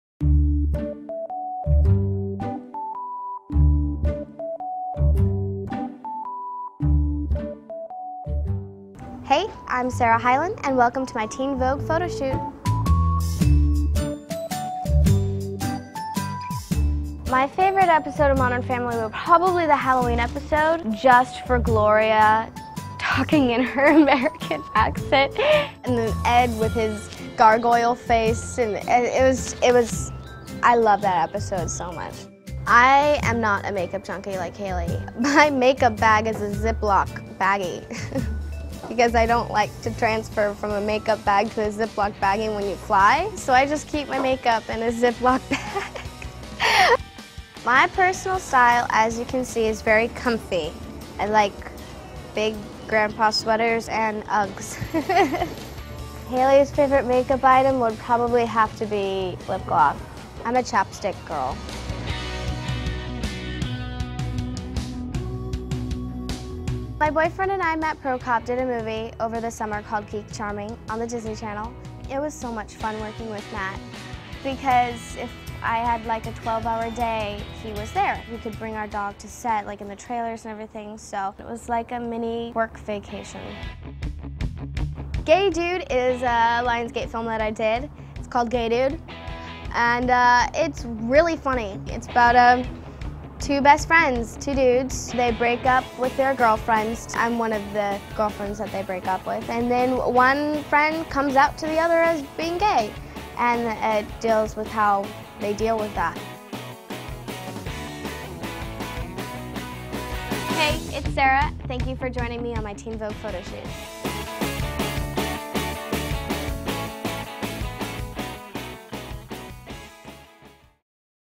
Behind the scenes with Sarah Hyland on the set of her Teen Vogue shoot.
Watch a bonus interview with Sarah Hyland and go behind-the-scenes of her Teen Vogue photo shoot.